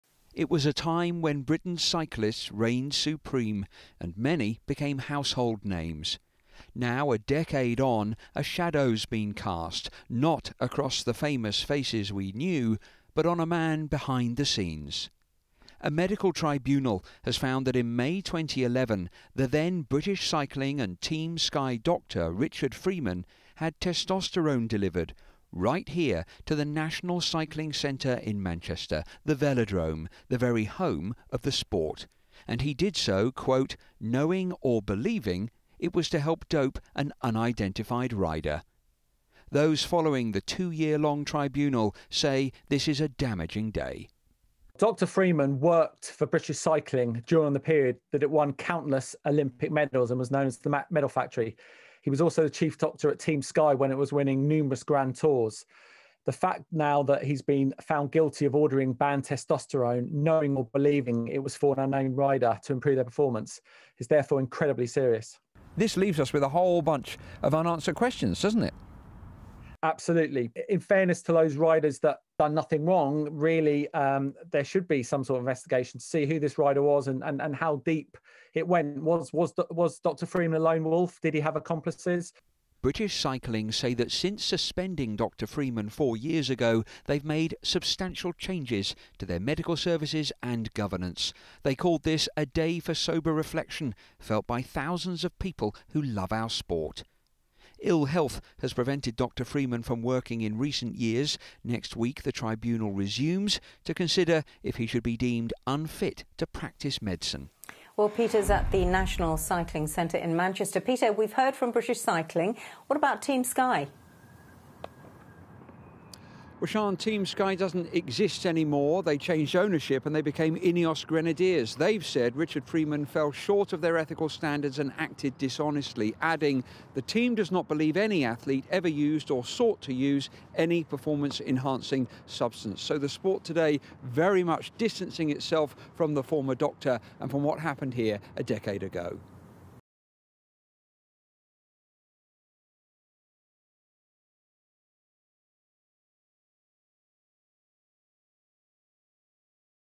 Ex British Cycling and Team Sky chief doctor Richard Freeman found guilty of doping charge | 5 News
► A key figure in Britain’s cycling successes has been found guilty of a doping charge by a medical tribunal.

Richard Freeman, chief doctor for British Cycling and Team Sky, ordered banned testosterone with the intention of doping a rider.

► Surge in bike sales as people keep active and avoid public transport in lockdown | 5 News